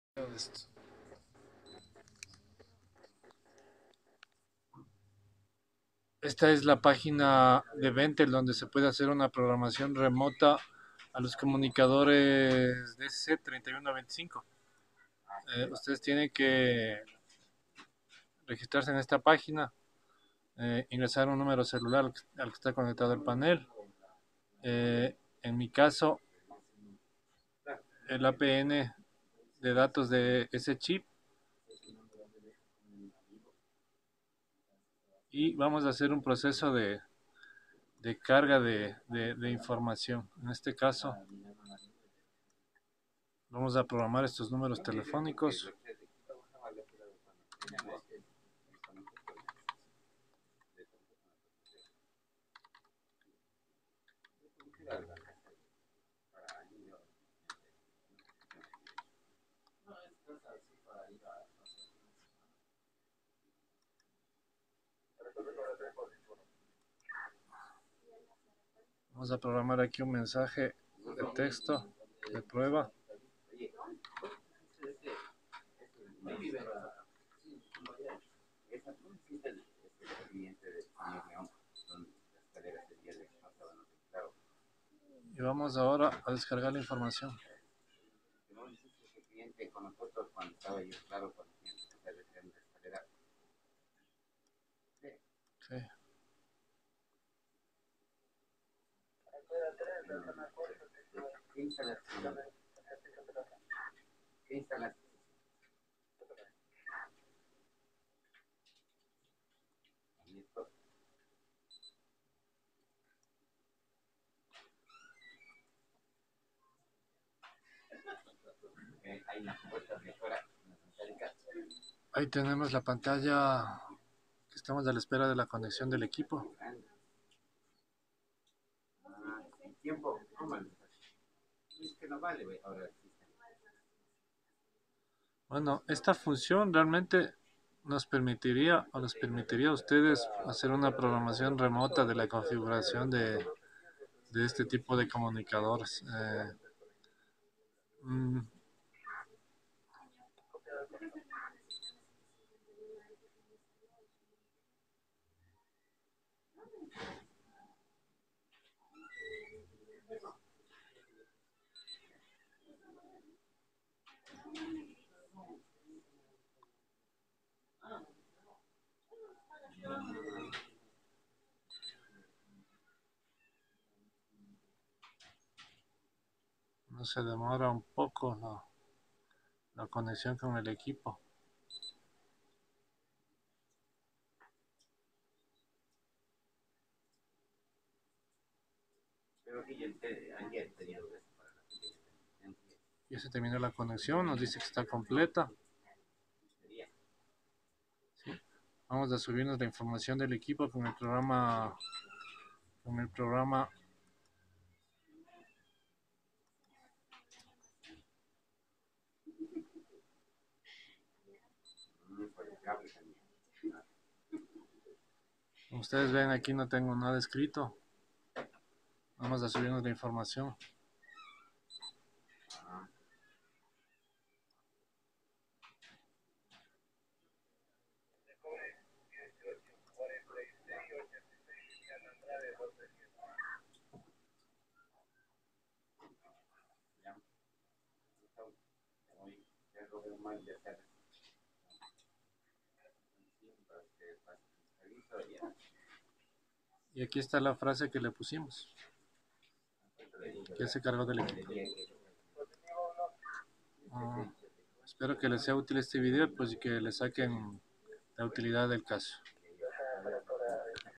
PROGRAMACION REMOTA GS3125
A TRAVES DE UNA OPCION WEB EN LA PAGINA DE BENTEL SECURITY SE PUEDE PROGRAMAR REMOTAMENTE ESTE EQUIPO